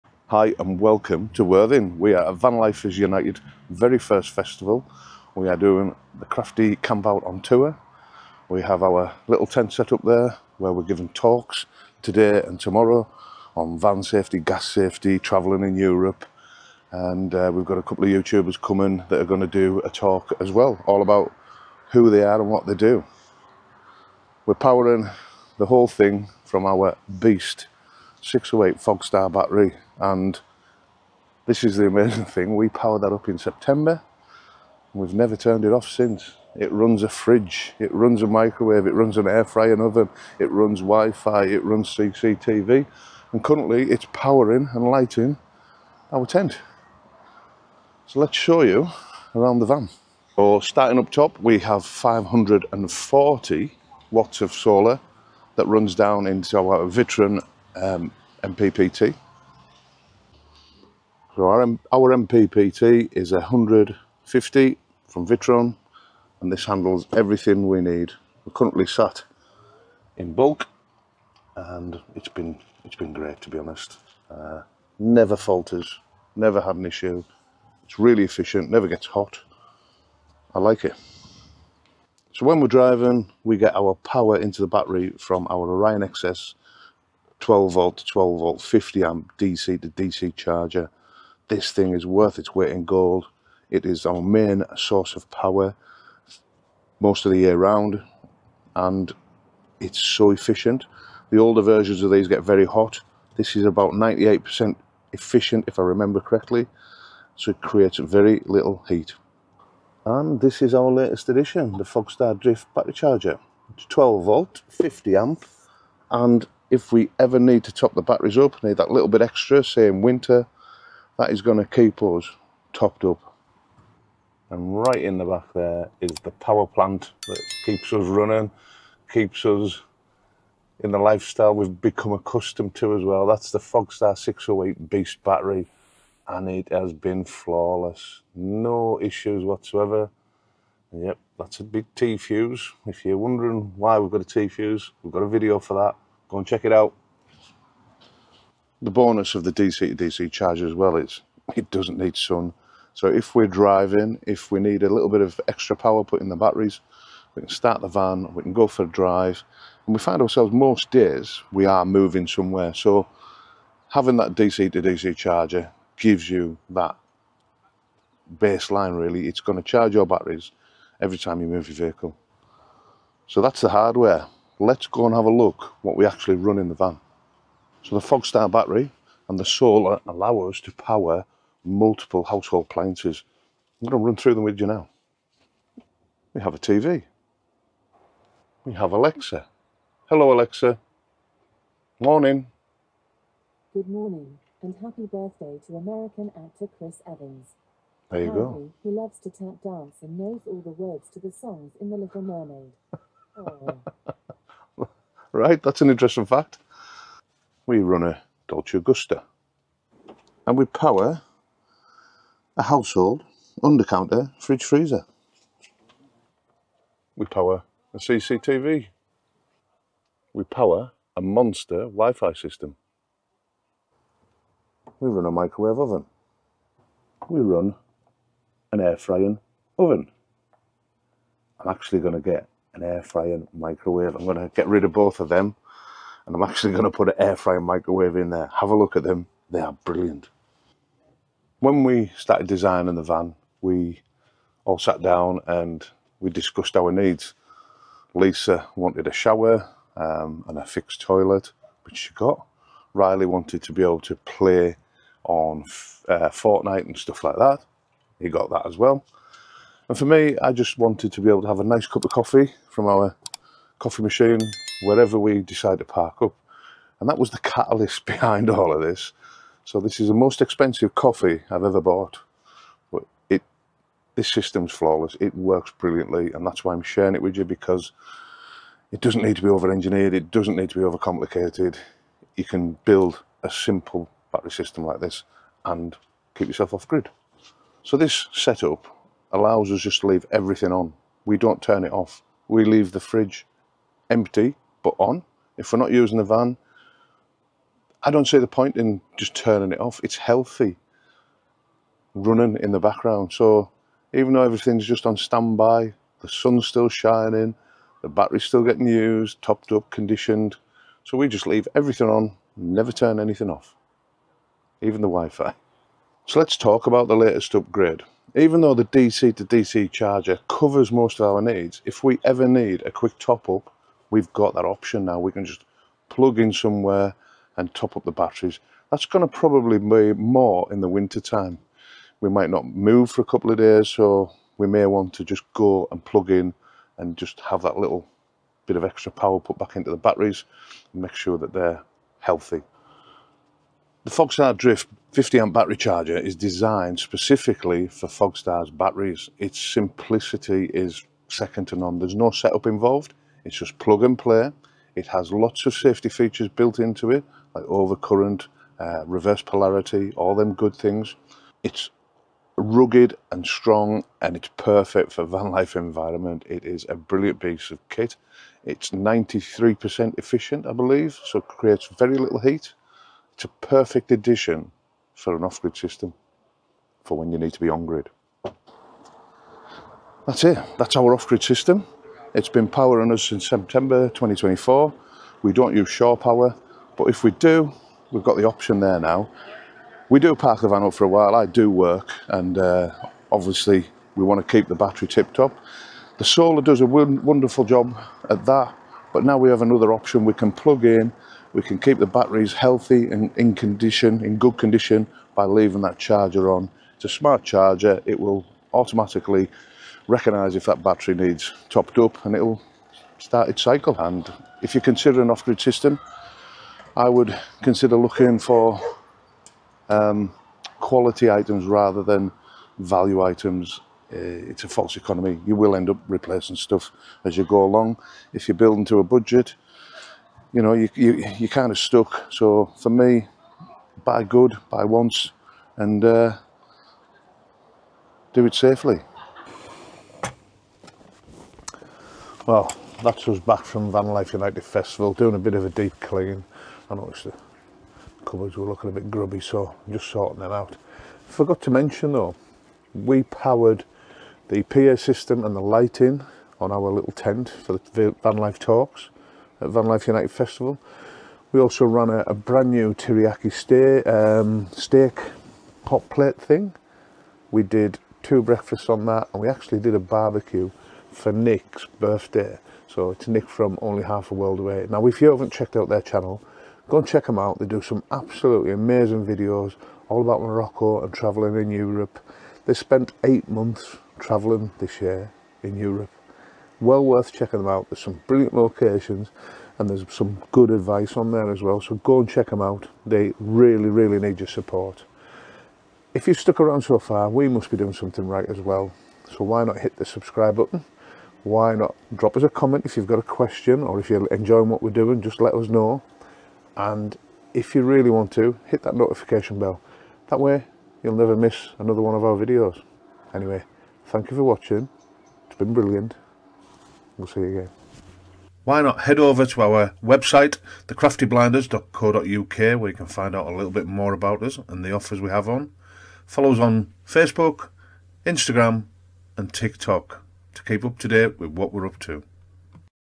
How We Power Everything Off-Grid | Fogstar Beast + Victron + Drift 50A Charger
This is the off-grid power setup that keeps us going – anywhere, anytime! 💪🔋
In this video, we dive into our 230v campervan electrical system designed for true off-grid living. With our 608Ah Fogstar “Beast” lithium battery, Victron 50A DC-DC charger, and Victron MPPT solar controller, we can easily power household appliances like:

✅ Full-size fridge
✅ Air fryer
✅ Microwave
✅ Wi-Fi router
✅ Laptop & editing gear and so much more

And now, we’ve added the brand-new Fogstar Drift 50A mains charger perfect for topping up from shore power when needed, giving us total energy flexibility on the road.

* Fogstar Energy 608Ah "Beast" Lithium Battery
* Victron Orion DC-DC 50A Charger
* Victron SmartSolar MPPT 100/50
* NEW Fogstar Drift 50A Charger (Mains)

If you’re building an off-grid rig or upgrading your camper’s power system, this setup is solid, scalable, and battle-tested for vanlife!

If you are enjoying the music in our videos, click on the link for loads more great music at Epidemic sounds